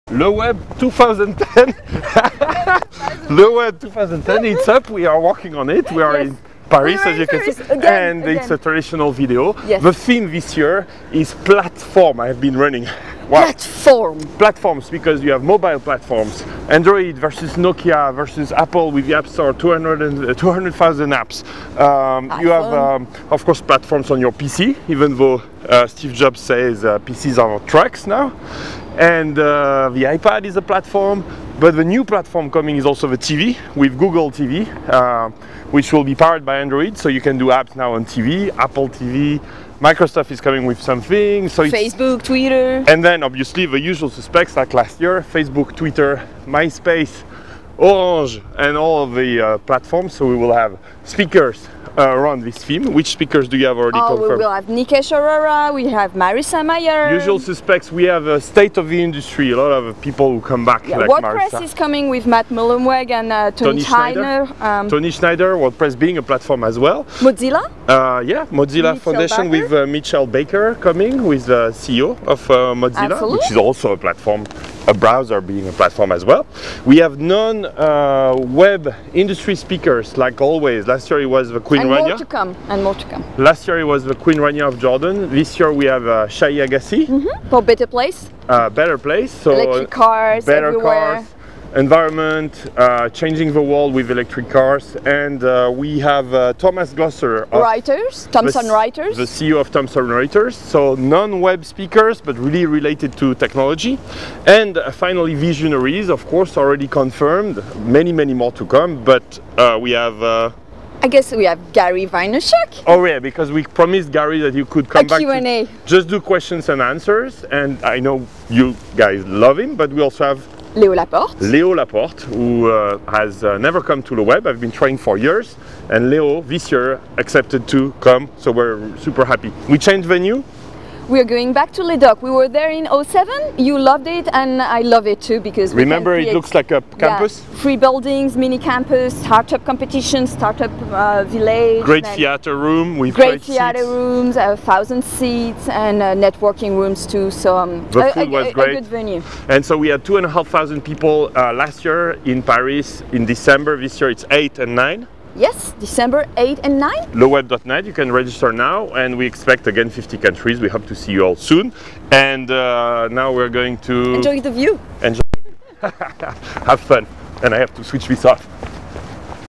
LeWeb 2010 theme and first speakers announced
Platforms is going to be our theme this year since everything is turning into a platform: social networks, mobile with iPhone and its 200,000 apps, Android, Blackberry and Nokia, of course